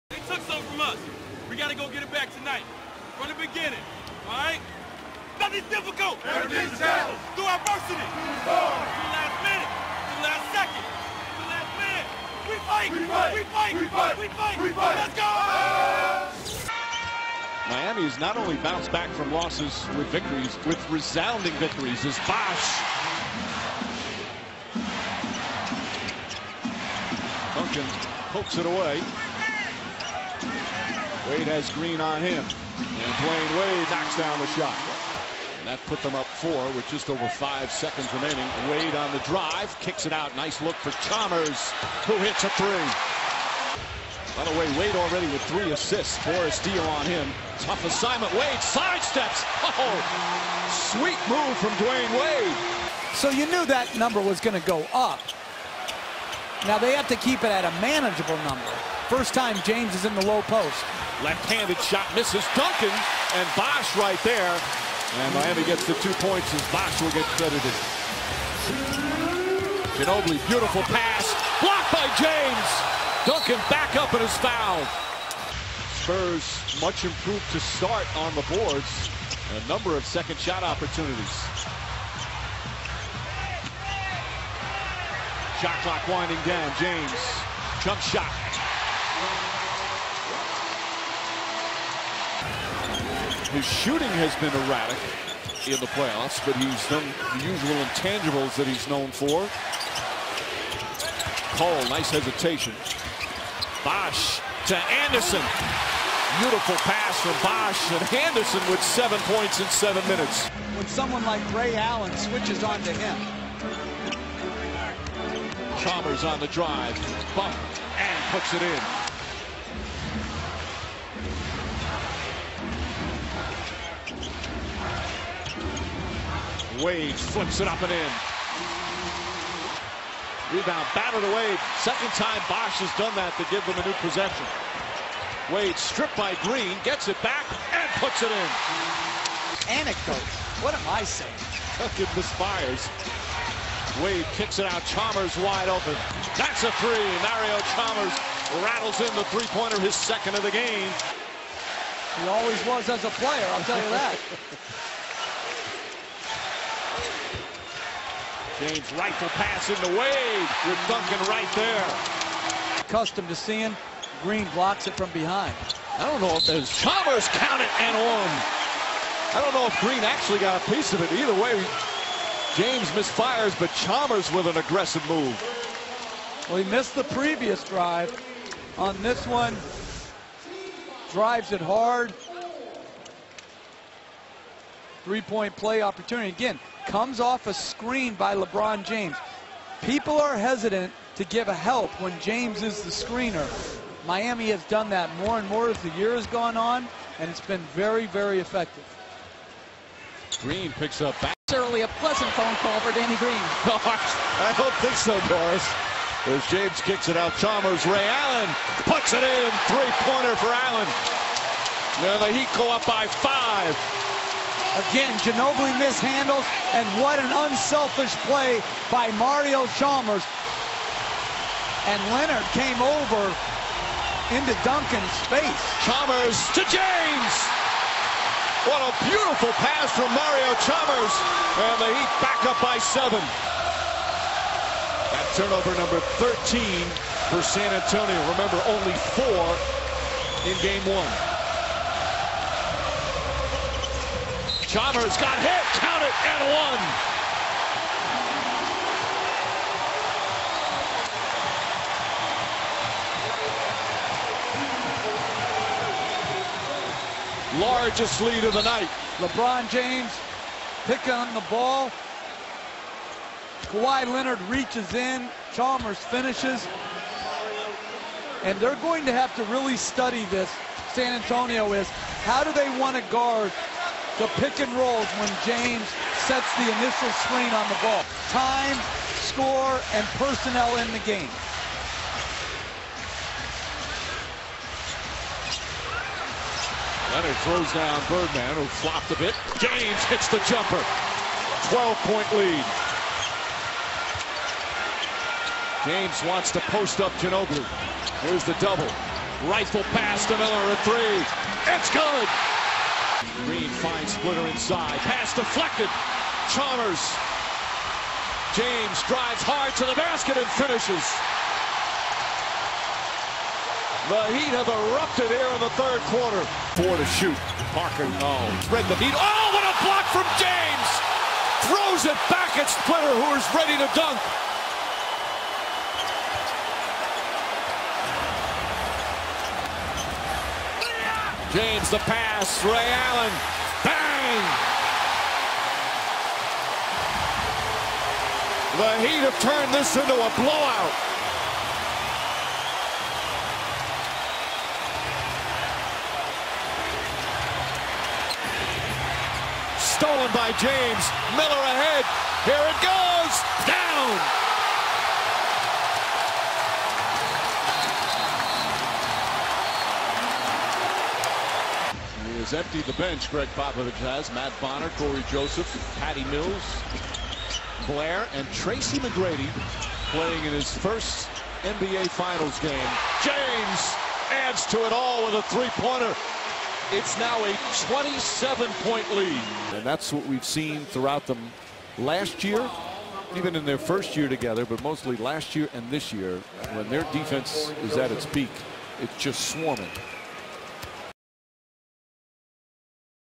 2013 NBA Finals Game 2 (Heat 84 - Spurs 103): Best of the Heat
Trailing 1-0 against the Spurs, it was revenge time for the Heat. Dwyane Wade (10 pts, 6 ast) and Chris Bosh (12 pts, 10 reb, 3ast) kept the team alive through the difficult 1st half. In the second,  Mario Chalmers (19 pts, 2 ast, 4 reb) sparked the Heat with an amazing performance and when Lebron James (17 pts, 7 ast, 8 reb) finally unleashed a flurry of shots, assists and an amazing block on Tiago Splitter, Miami cruised to a 84-103 win.